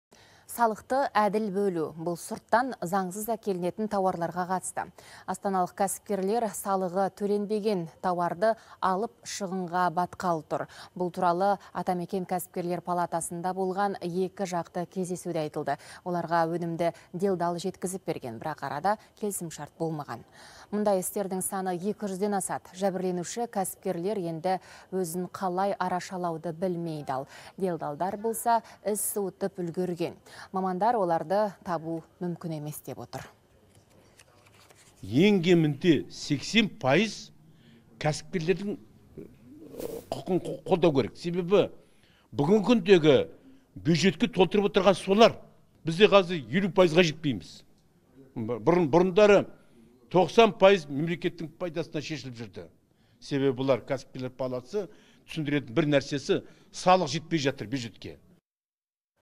Астаналық кәсіпкерлер шығынға батқалы тұр
Бұл туралы Атамекен кәсіпкерлер палатасында болған екіжақты кездесуде айтылды.